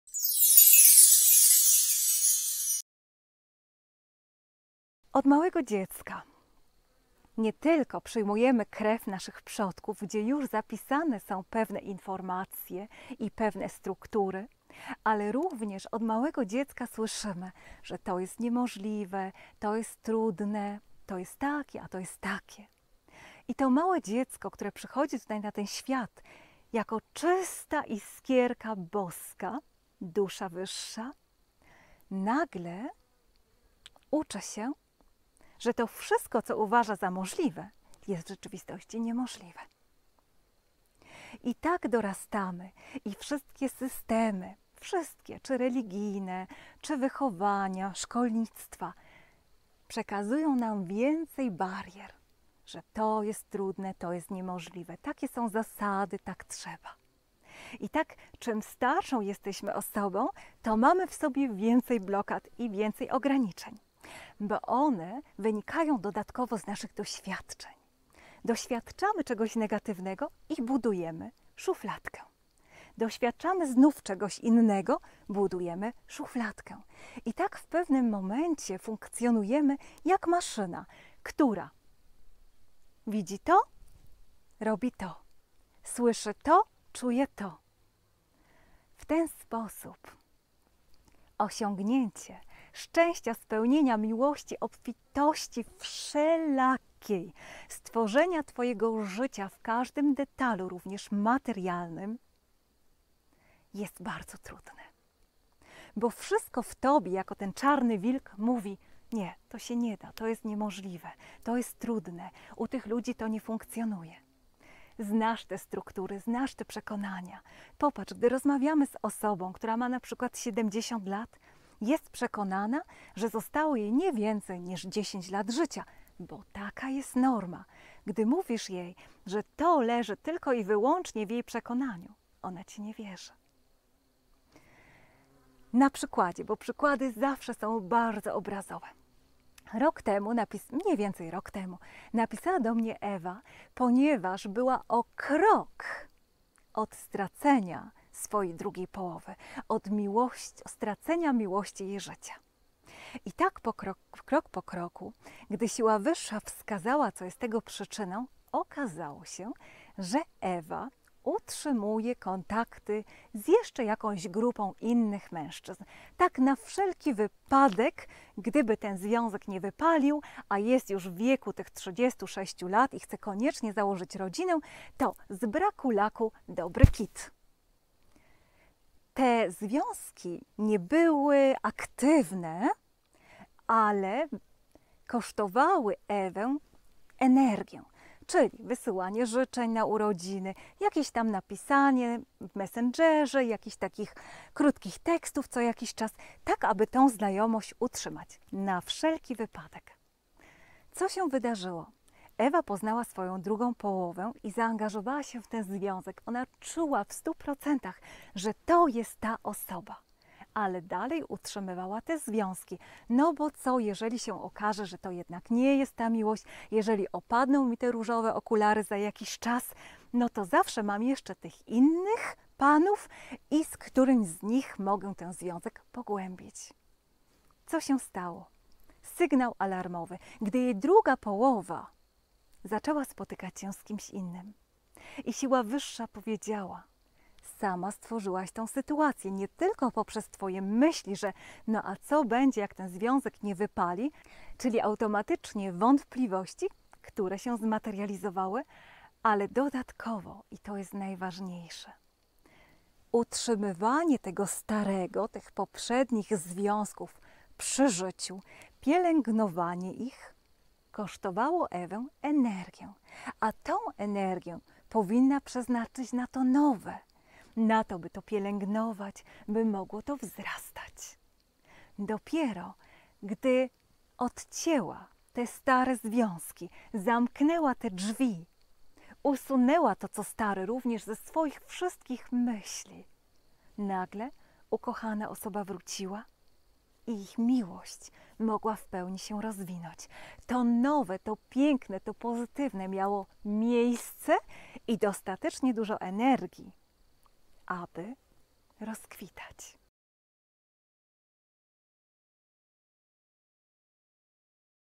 791. Jakie szkodliwe przekonania, utrudniają Ci życie? Czas, aby się ich pozbyć! inspiracja nr 38 ✨
Ten wykład w całości znajdziesz  w Akademii Omen Sententia

📳Moje Strony na Facebook:
▶Magiczny Świat:
▶Jak być prawdziwą kobietą?